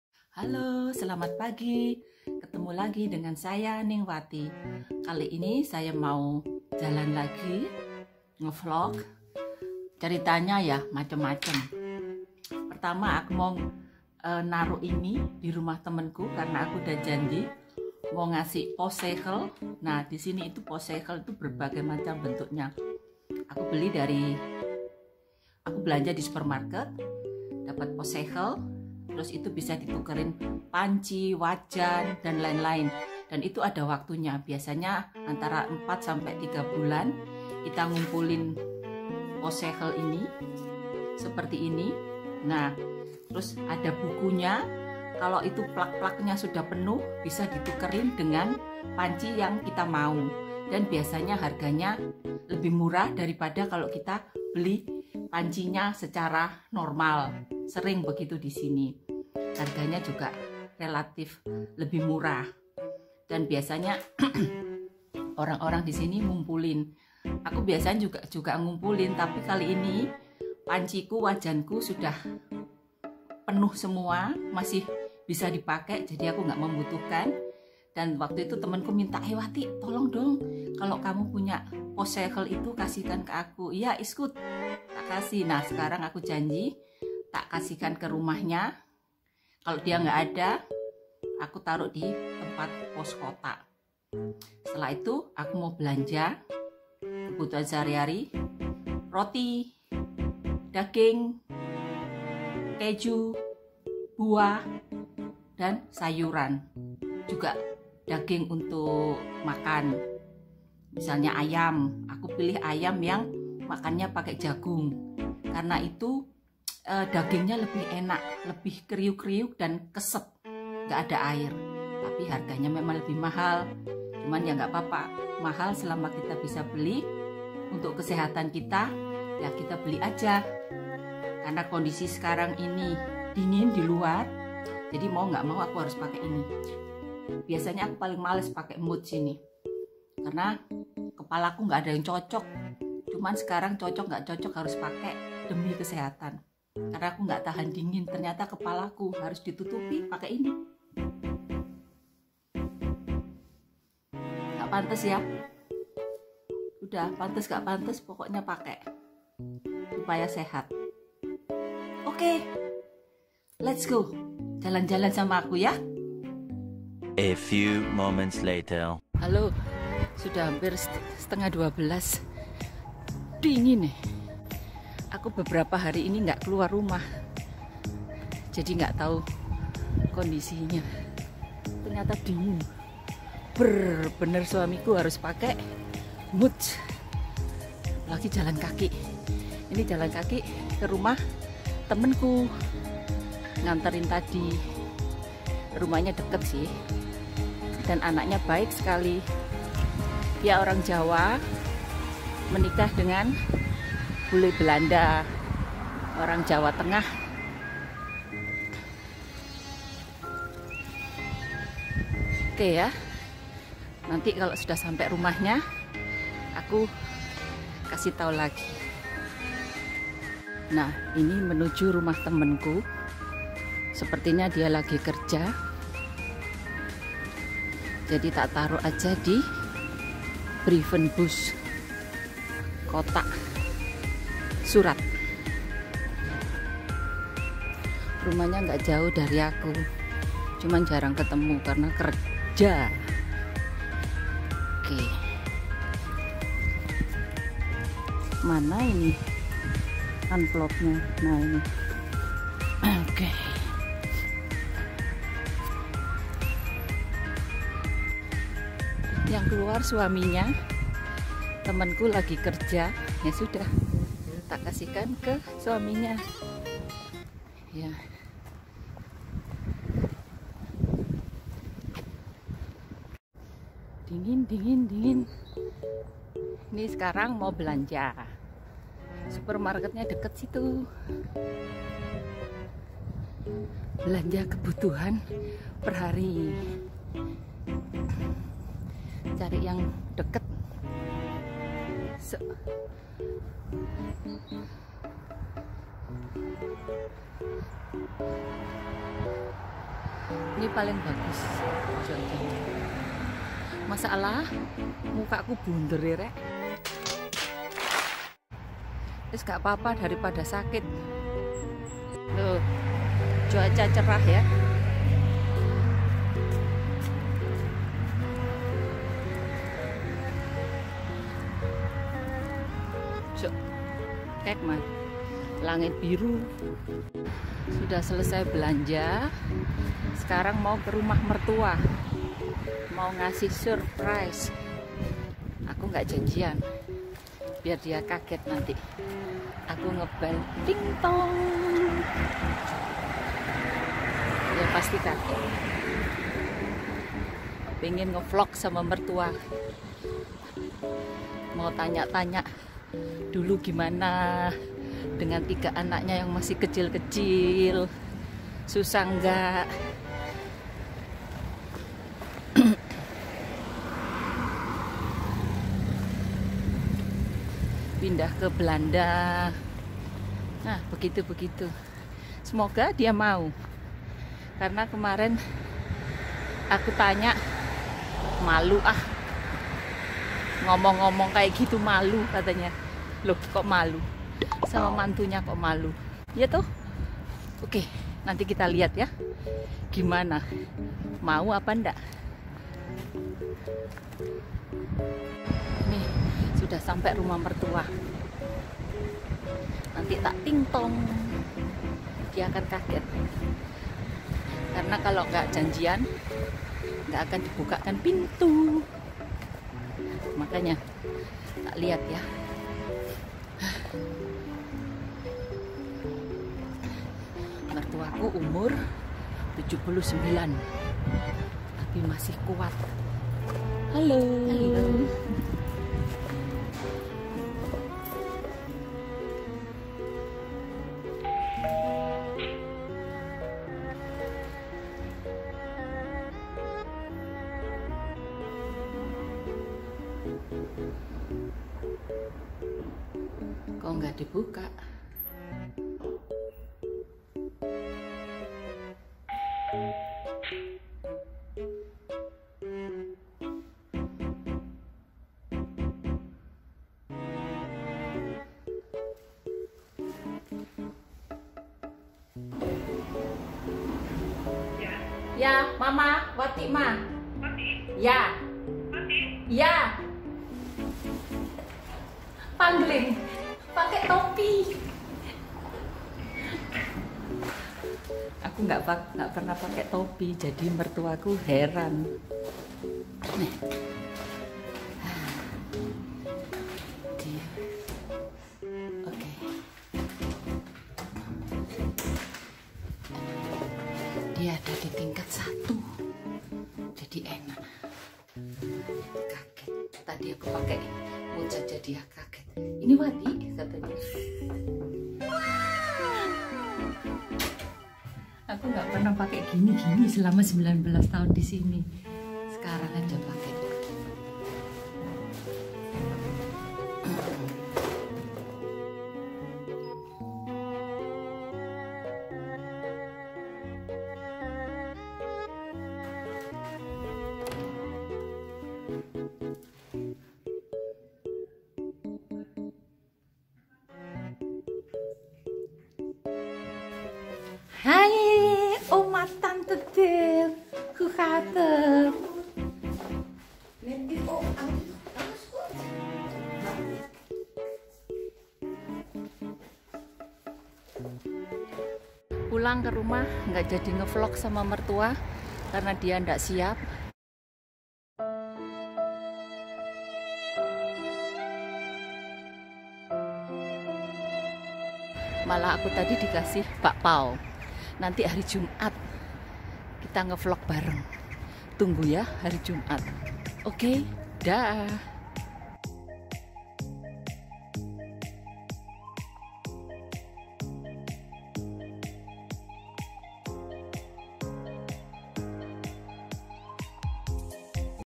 JALAN-JALAN KELUAR, ADEM REK...
Kali ini aku mau jalan-jalan ke berbagai tujuan, mulai dari ke rumah temanku, belanja, dan ke rumah mertua. Yuk ditonton keseruanku selama jalan-jalan kali ini. THANK'S :)